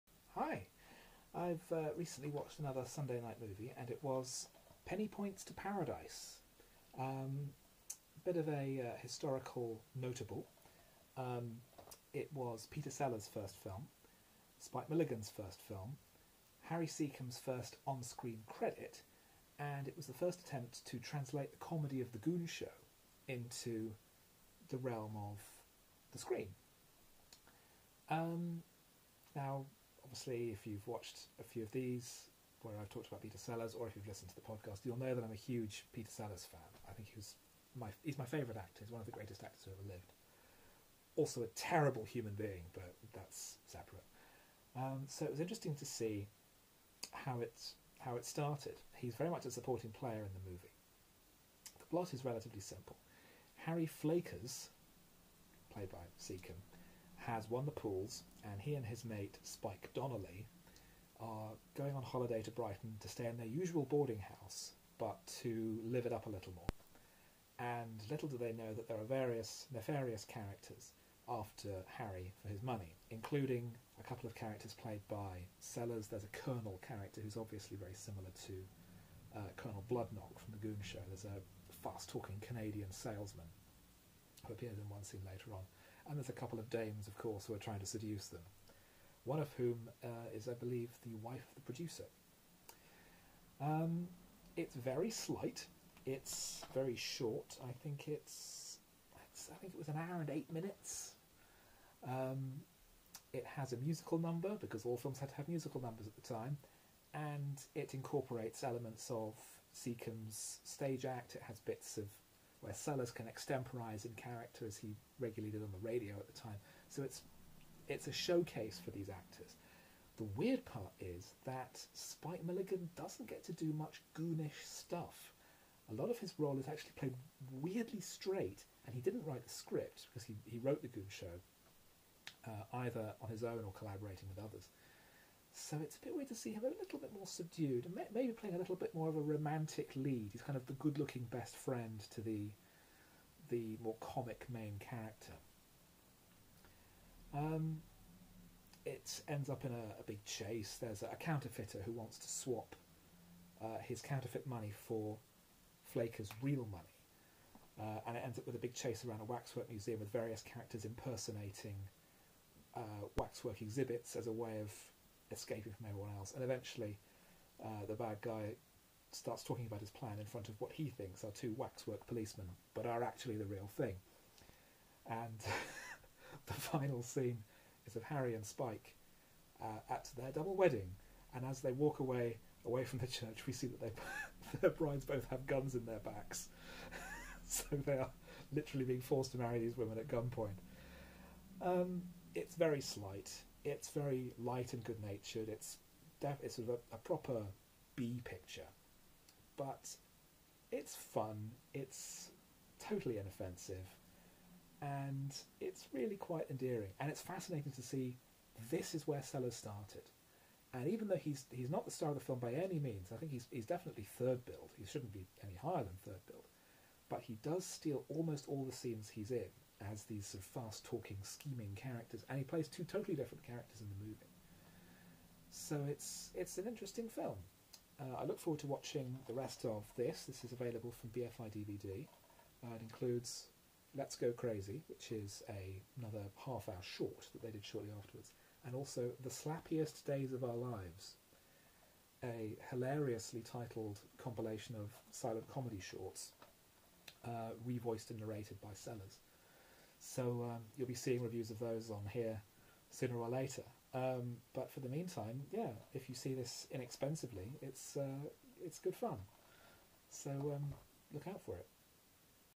First Takes - Penny Points to Paradise (1951)
Jeremy reviews Peter Sellers's film debut abs the first attempt by the Goons to conquer the cardboard cinema.